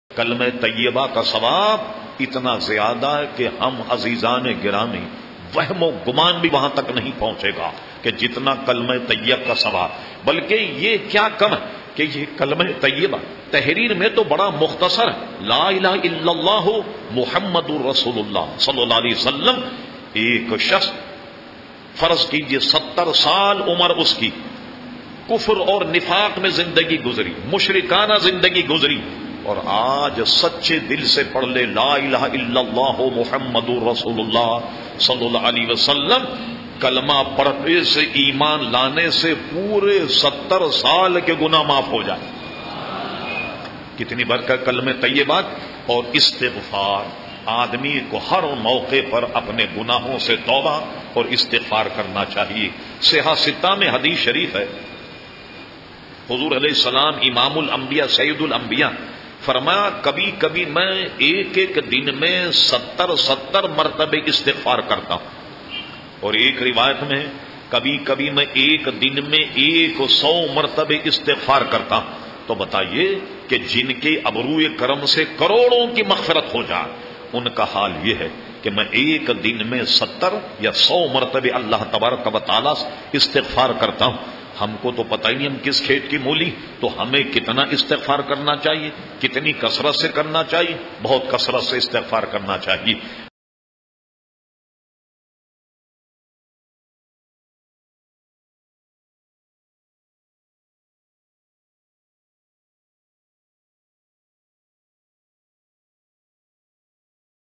Kalma-e-Tayyaba ki Barakatein
کلمہ طیبہ کی برکتیں
Voice:
Hazrat Allama Syed Shah Turab-ul-Haq Qadri Alaih Rehma